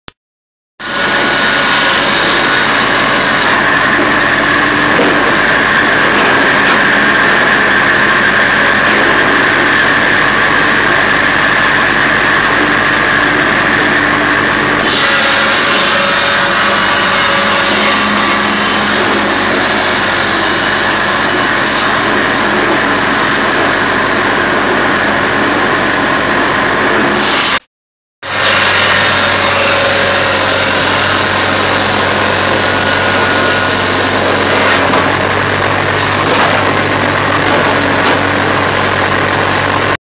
Oak Beams.mp4
The Roots and Routes team making oak beams with their big boy toy. We have all manner of aggregate and building materials available to buy, including firewood and railway sleepers.